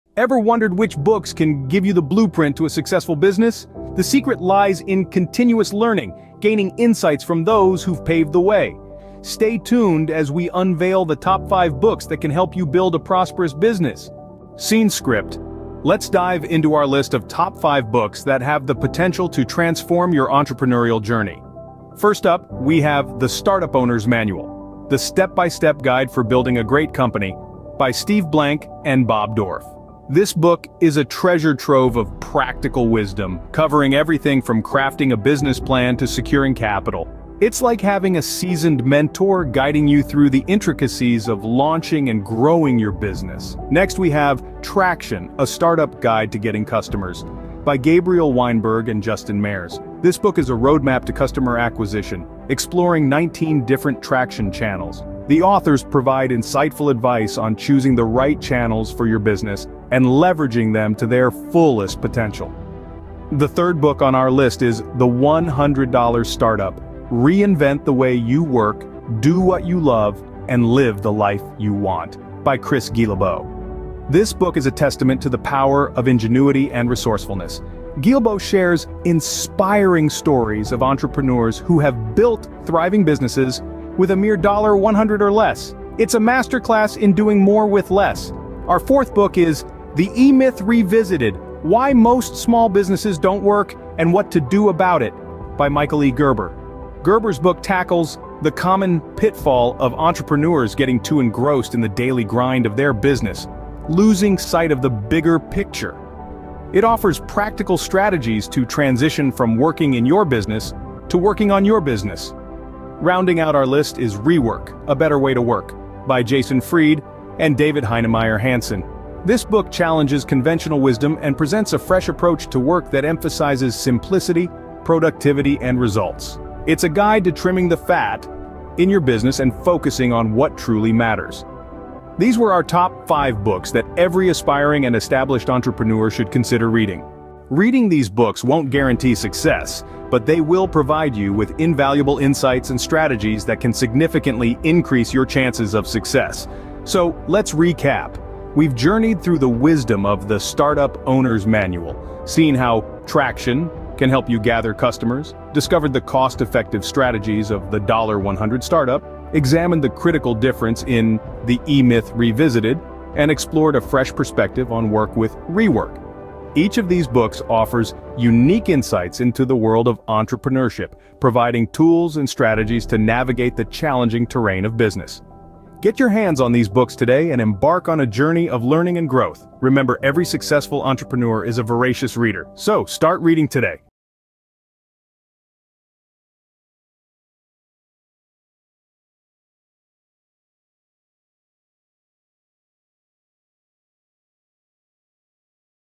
top 5 best how-to make successful business books on Amazon
1. The Startup Owner's Manual
2. Traction
3. The $100 Startup
4. The E-Myth Revisited
5. Rework: A Better Way to Work

These five books are essential reading for anyone who wants to start or grow a successful business. They're full of practical advice and insights that can help you avoid pitfalls, overcome challenges, and achieve your goals.